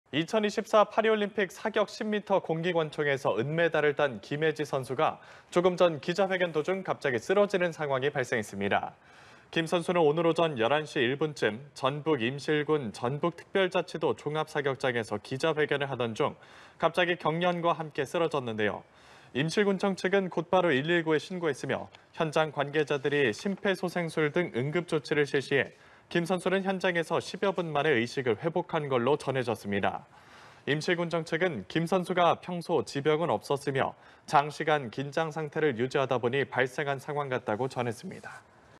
사격 은메달 김예지, 기자회견 중 쓰러졌다 회복 / 연합뉴스TV (YonhapnewsTV)
사격 은메달 김예지, 기자회견 중 쓰러졌다 회복

2024 파리 올림픽 사격 10m 공기권총에서 은메달을 딴 김예지 선수가 조금 전 기자회견 도중 갑자기 쓰러지는 상황이 발생했습니다.

김 선수는 오늘 오전 11시 1분쯤 전북 임실군 전북특별자치도 종합사격장에서 기자회견을 하던 중 갑자기 경련과 함께 쓰러졌는데요.

임실군청 측은 곧바로 119에 신고했으며, 현장에서 관계자들이 심폐소생술 등 응급조치를 실시해 김 선수는 현장에서 10여분 만에 의식을 회복한 것으로 전해졌습니다.

임실군청 측은 김 선수가 평소 지병은 없었으며 장시간 긴장 상태를 유지하다보니 발생한 상황 같다고 전했습니다.

(끝)